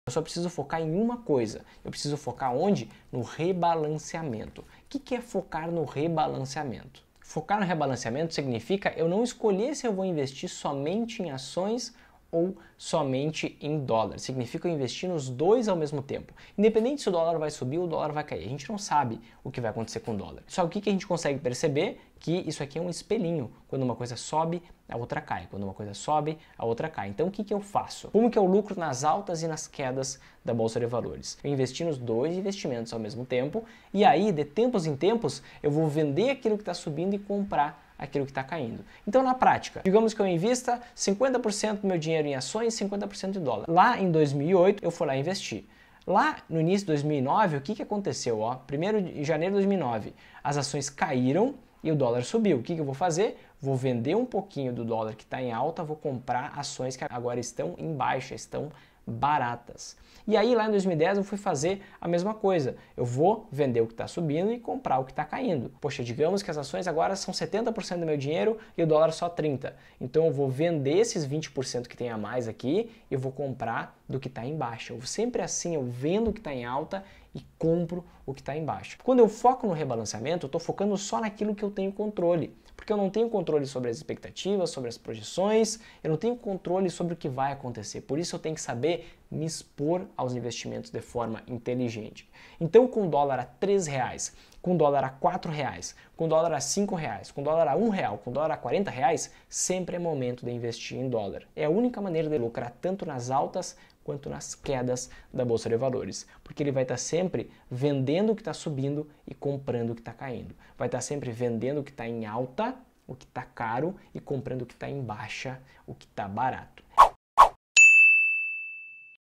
A ÚNICA MANEIRA DE LUCRAR DURANTE AS QUEDAS DA BOLSA | Investidor Sempre em Alta
Essa é a única maneira de lucrar durante as quedas da Bolsa de Valores.